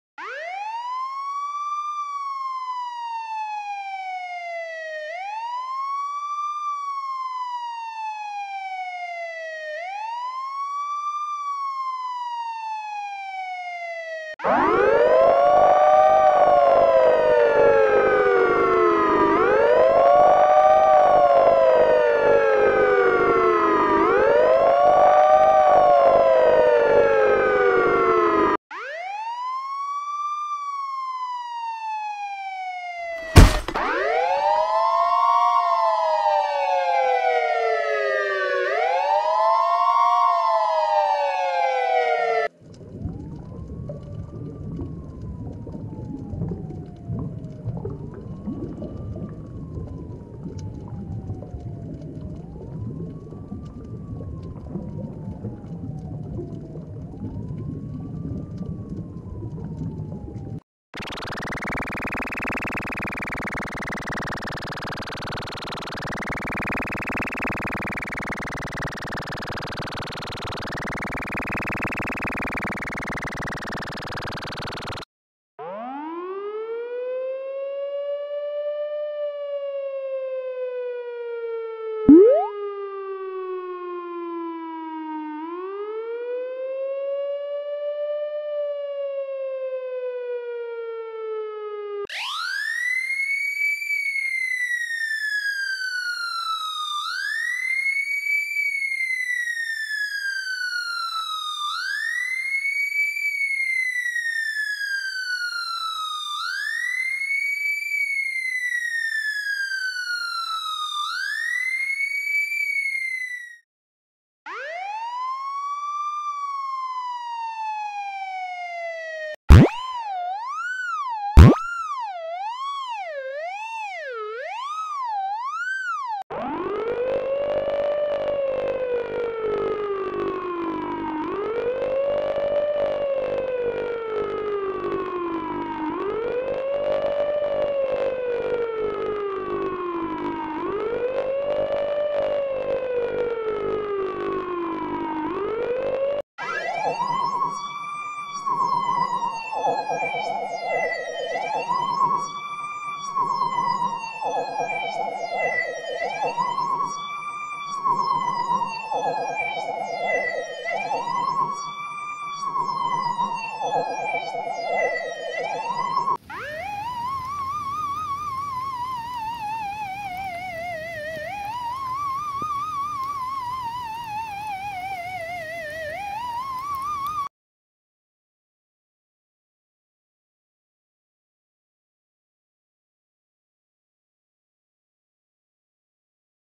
Police Car Sound Variation part-2 en 130 seconds
11 Police car Siren Sound Variation part-2
 Police Car Siren Sound Variations
Video Editing time for this video : 9 hours.
All materials for this video are taken from open sources and are free.